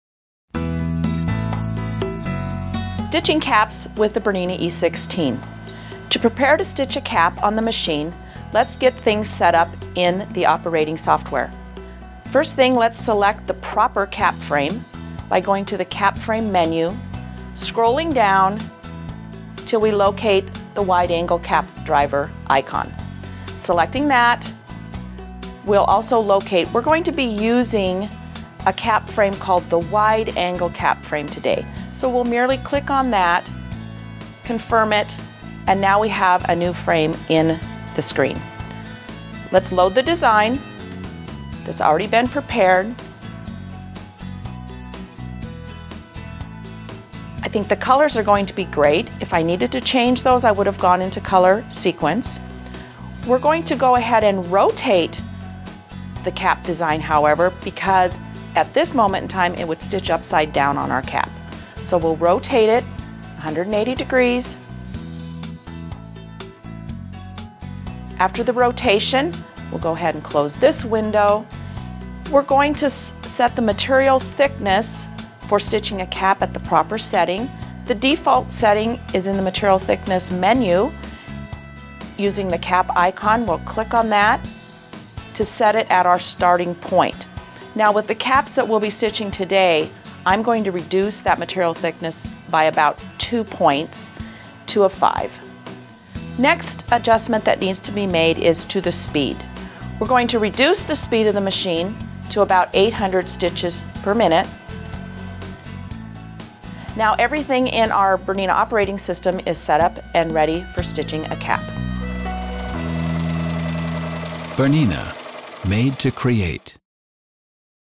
BERNINA E 16 Tutorial: Stitching a Cap – Computer
The BERNINA E 16 takes embroidery to a new level with a 16 needle embroidery machine.

In this video tutorial we will show you step by step how to prepare to stitch a cap on the BERNINA E 16 operating software.

More how-to videos on the BERNINA E 16 can be found in the playlist.

More information about the BERNINA E 16 is available from your BERNINA E 16 dealer